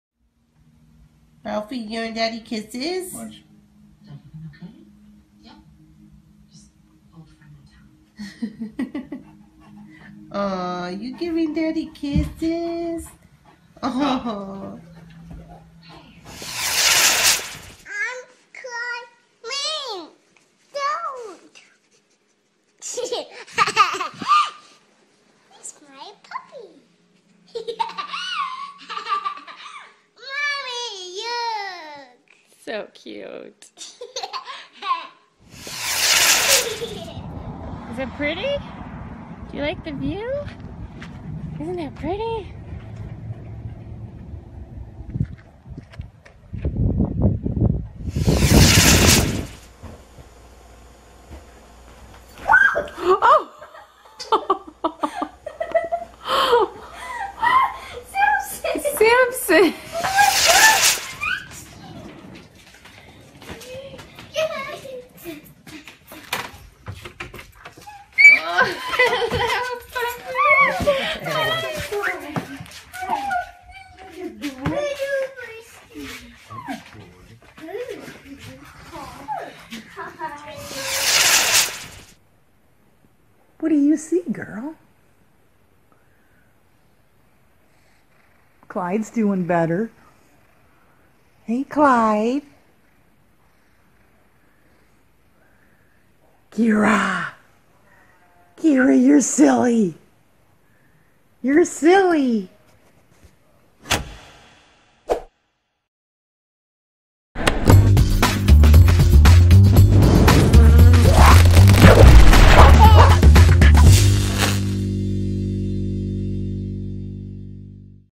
Newfoundlands Are Big Dogs With BIGGER Hearts!
Rumble Viral showcases a collection of Newfoundland dogs that will make you fall in love with this amazing breed. Check them out and let us know which one's your favorite!